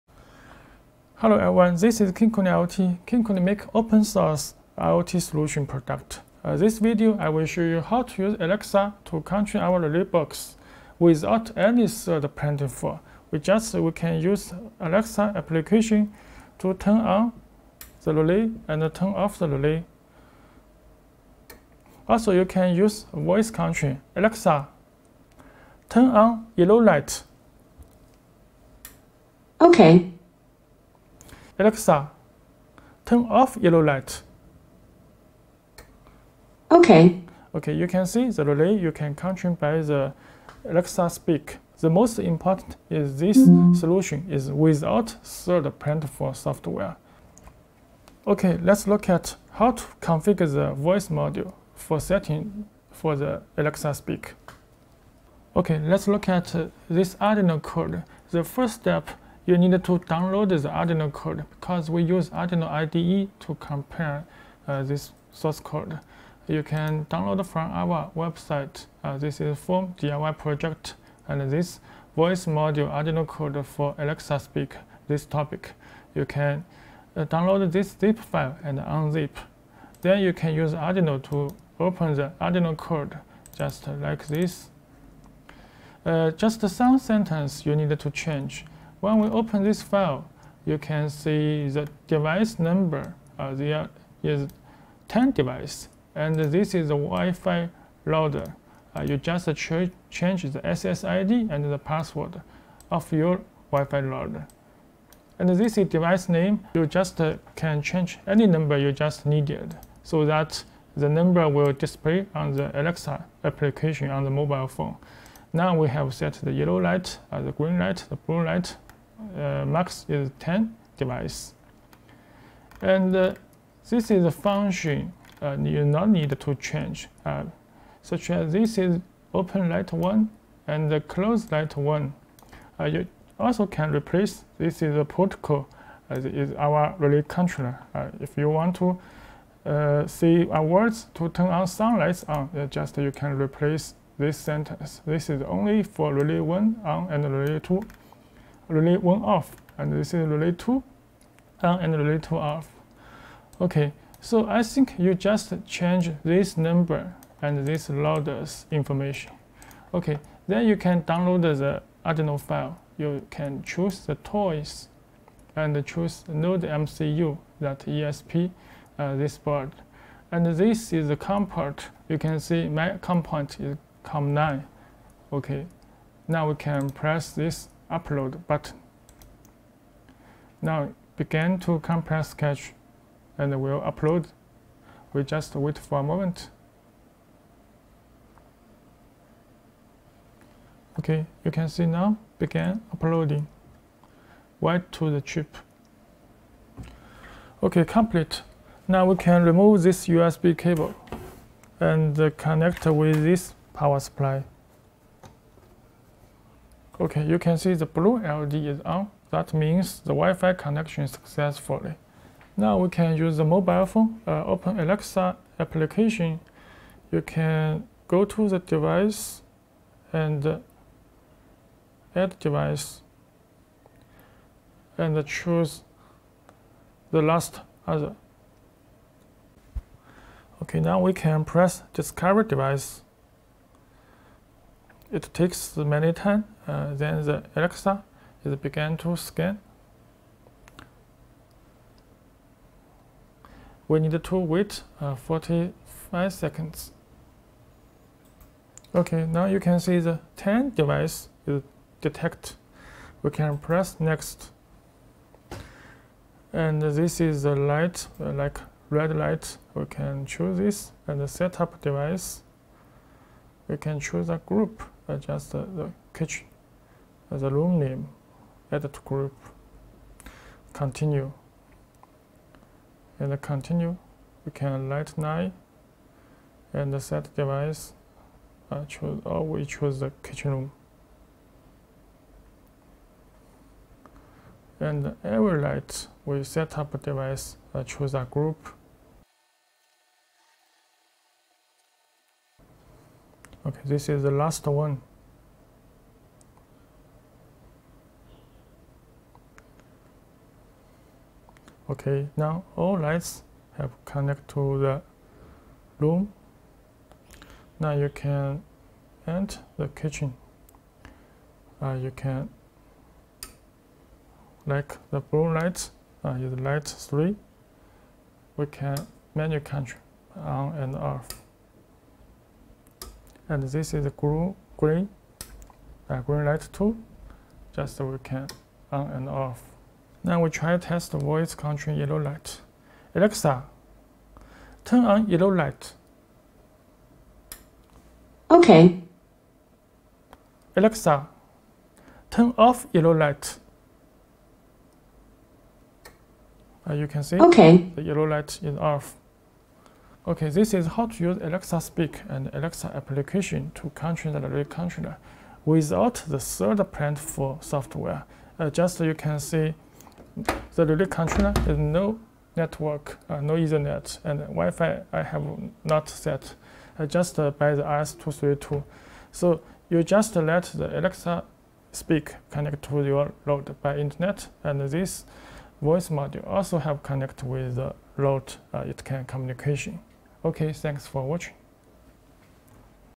voice control relay by Alexa without any 3rd party platforms ESP8266
This is arduino code for voice module (ESP8266) control by Amazon alexa. it not need use sinric or any third platform. just voice module connect to alexa cloud server directly.
MAX support device number :10pcs. if want to support more, need change the code, or you can use google find out some code solution.

make sure install espalexa.h library file on arduino IDE.